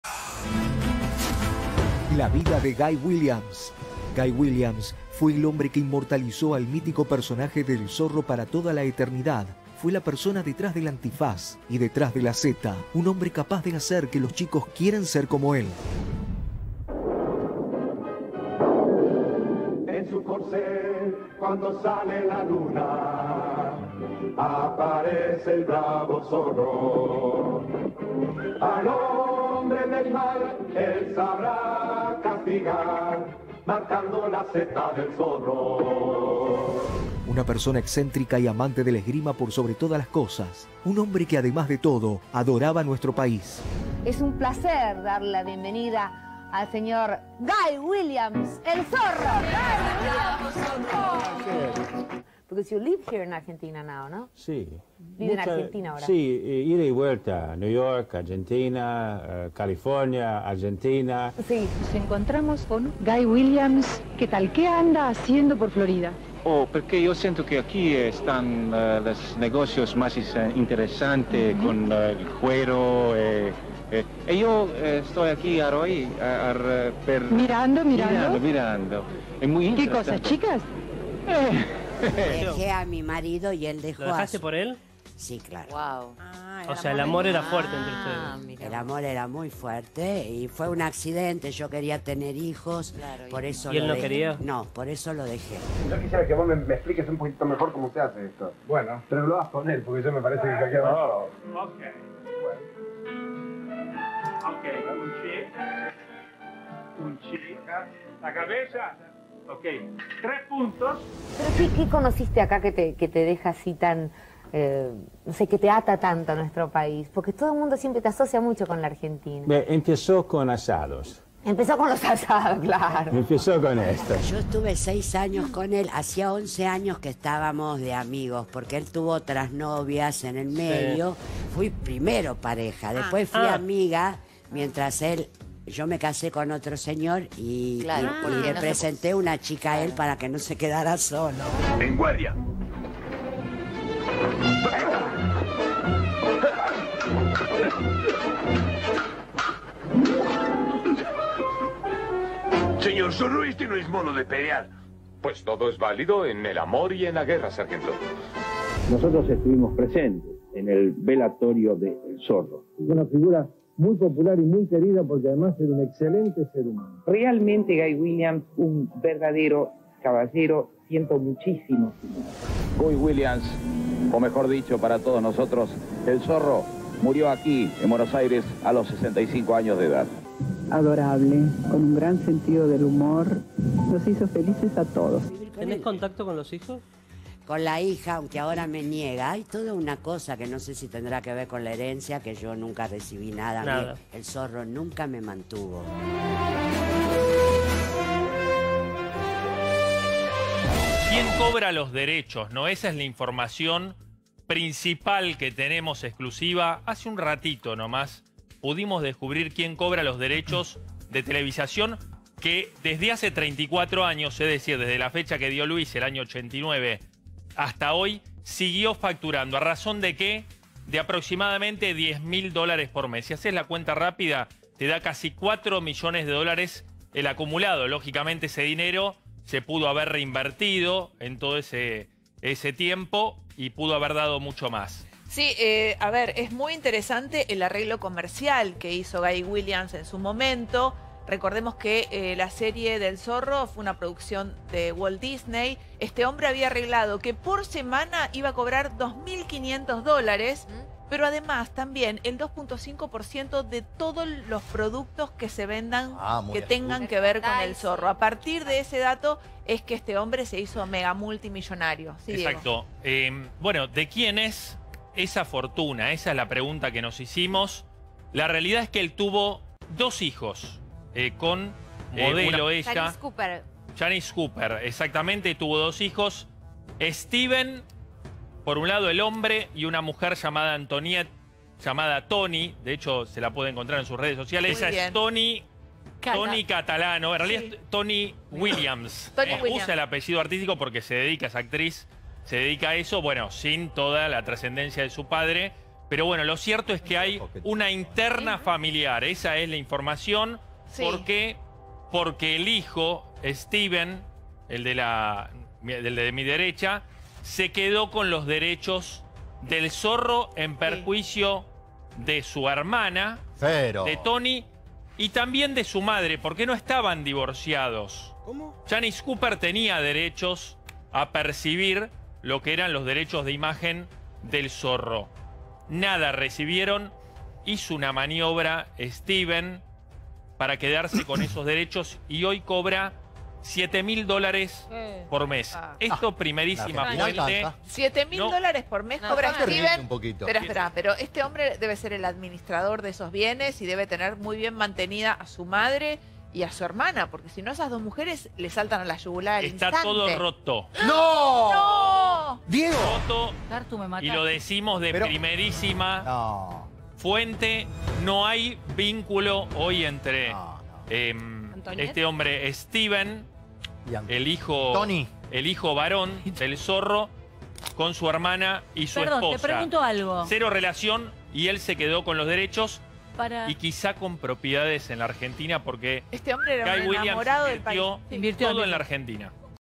💣Traición en la familia de "EL ZORRO": uno de sus hijos se queda con todo😳
-Guy Williams fue un conocido actor estadounidense, uno de sus mayores éxitos fue la serie de "El Zorro".
Guy tuvo dos hijos, Steven Catalano y Toni Catalano.
Luego de su fallecimiento en el año 1989, ha seguido acumulando fortuna.
-Steven, su hijo se habría quedado con toda la fortuna dejando fuera a su hermana.